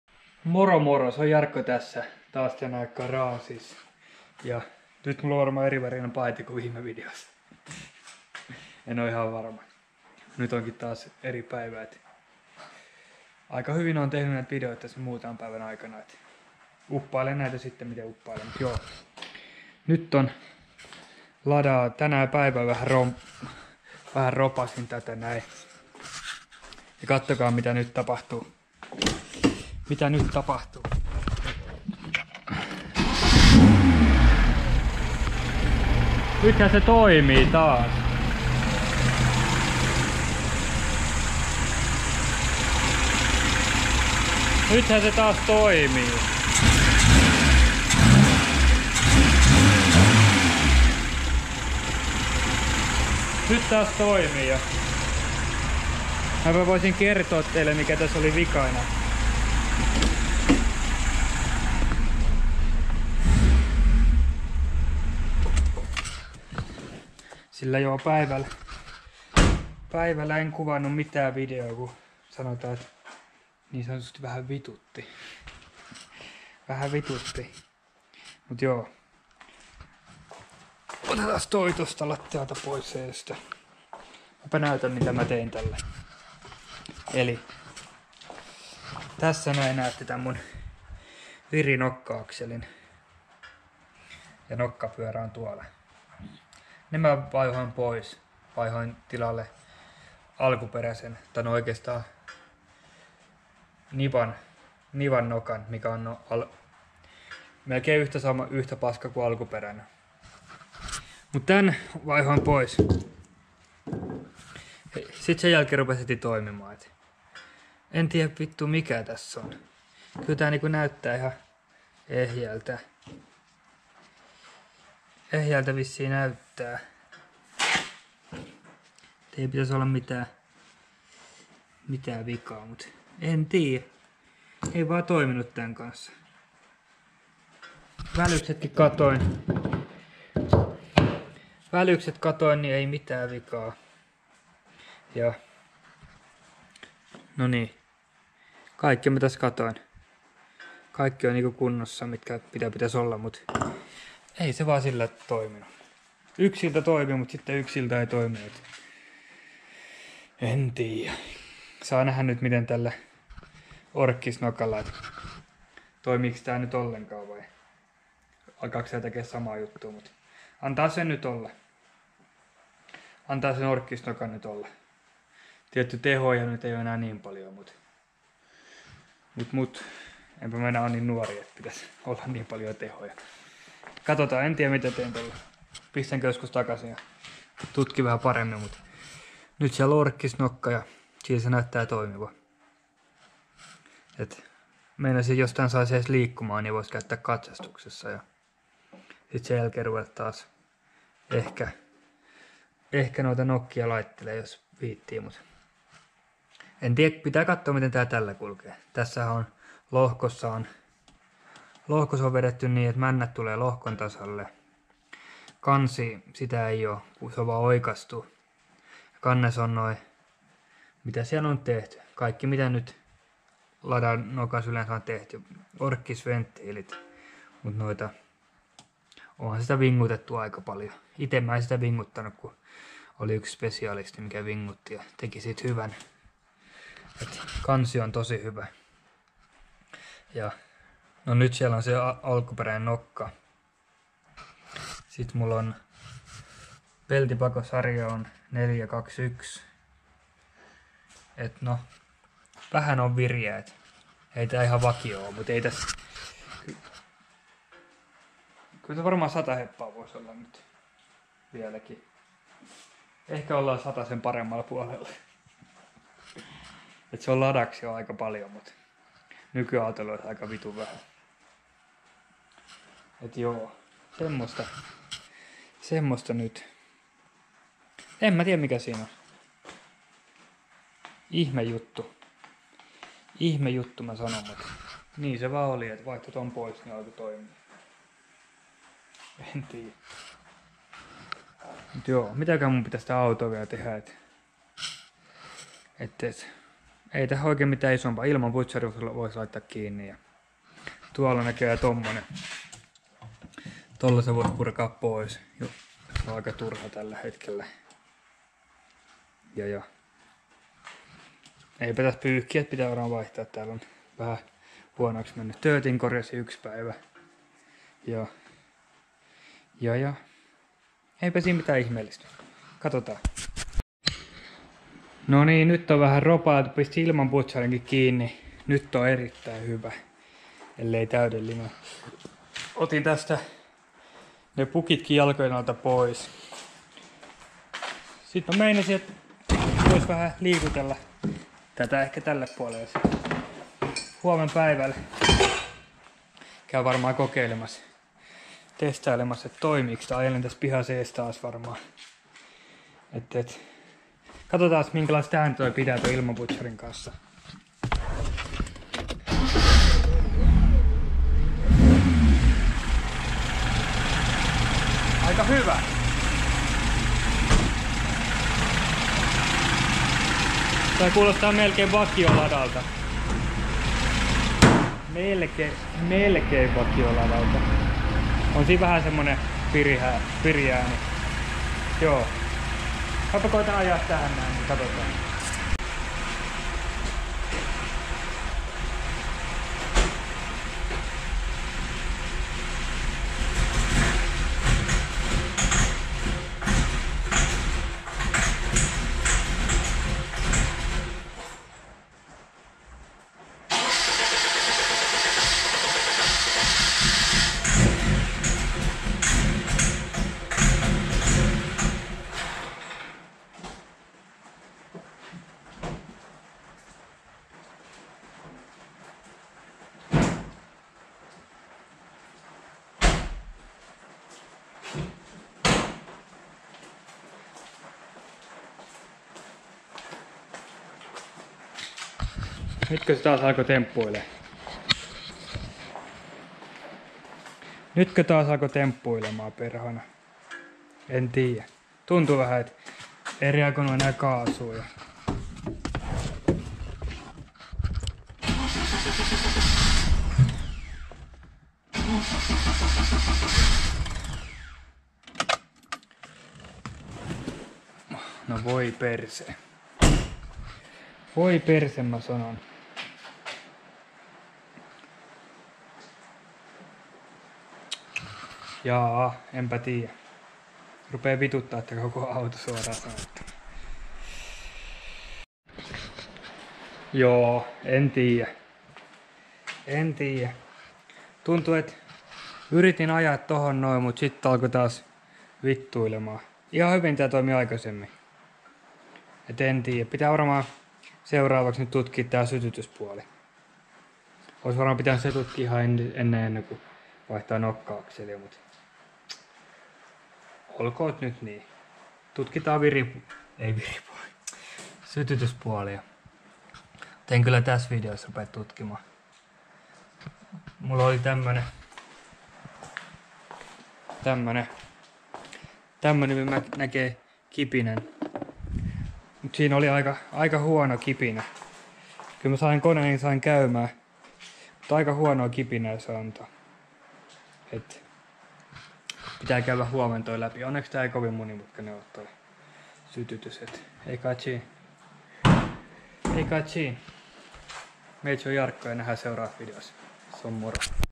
Auto taas käy mut ei sit käykkää (2102 valkonen farmari Lada)
Lähti käymää mut onneks ei lähtenykkää